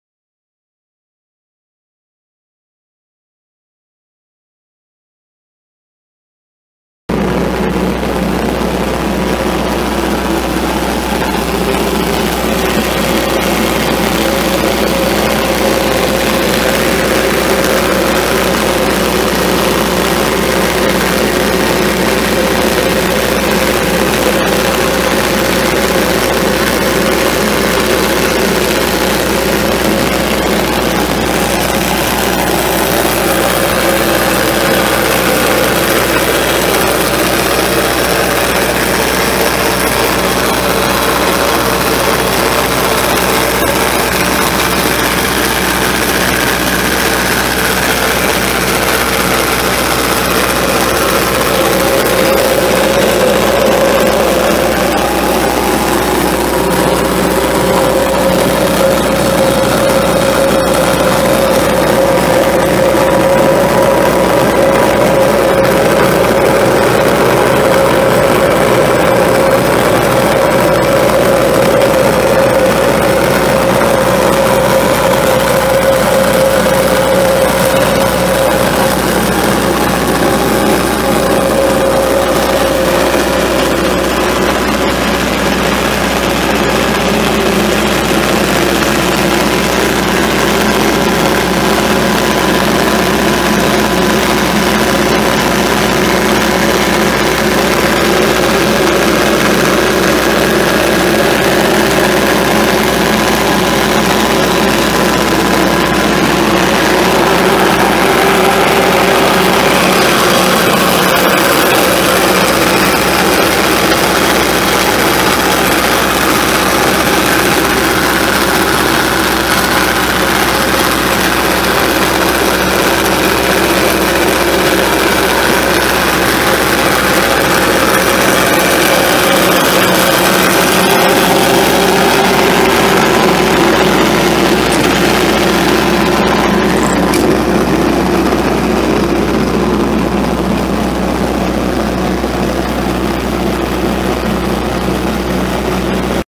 Caterpillar generator for sale | no-reserve Internet auction July 6, 2017
Caterpillar generator
3,282 hours on meter
Caterpillar 3054 four cylinder turbo diesel engine
Serial 4ZK04268
Generac 32145S generator
Electric start
Control panel
Fuel tank
Single and three phase
Serial 2021761
Safety chains
2 5/16" ball hitch
Surge brakes
ST205/75R14 tires
Unit# 30222